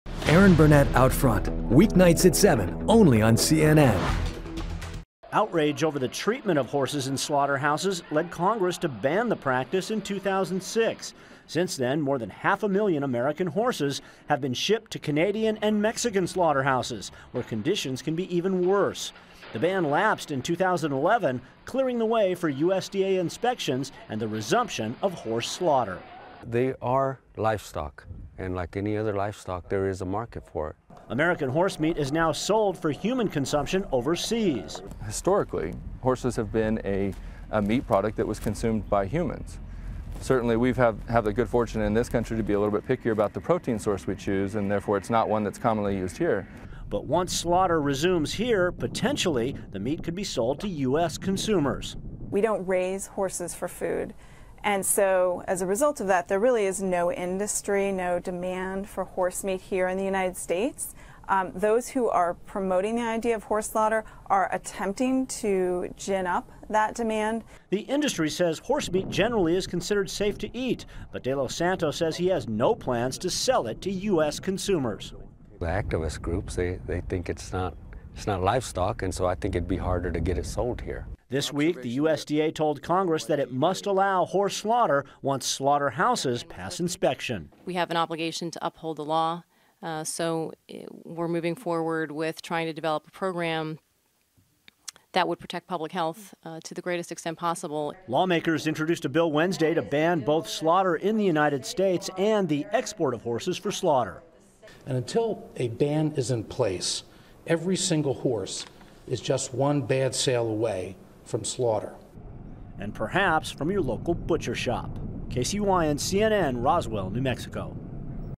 Horse slaughterhouse: No plans to sell in U.S.
All eyes are on a New Mexico slaughterhouse that plans to produce horse meat. For more CNN videos, visit our